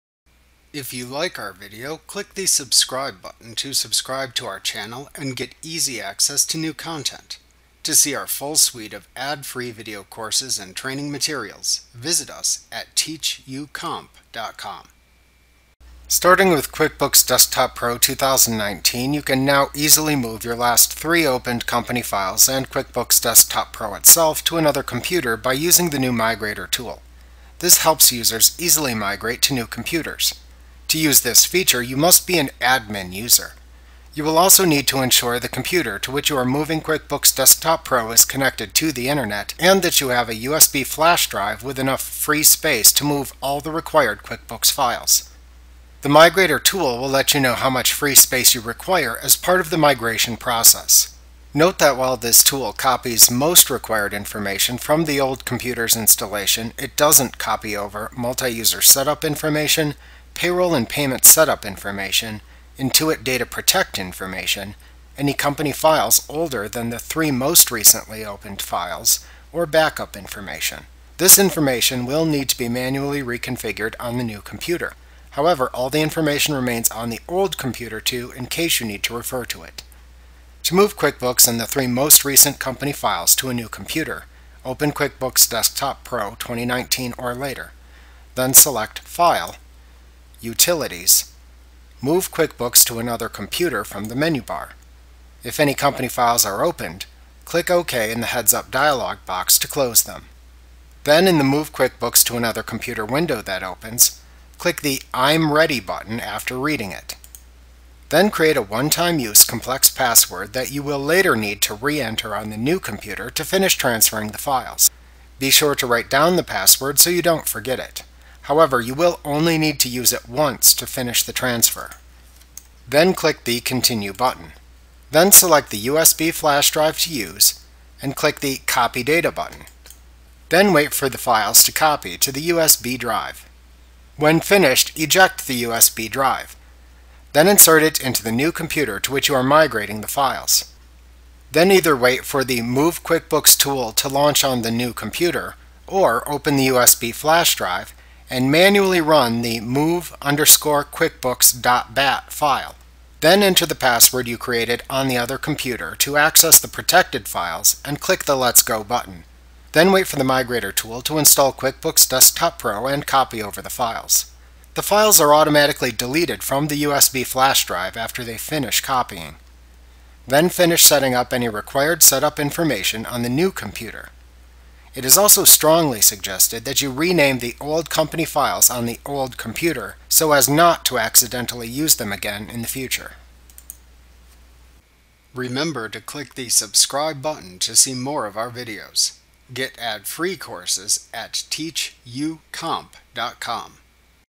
QuickBooks Desktop Pro 2021 Tutorial Move QuickBooks Desktop Using the Migrator Tool Intuit Training
Learn about Moving QuickBooks Desktop Using the Migrator Tool in Intuit QuickBooks Pro 2021 with the complete ad-free training course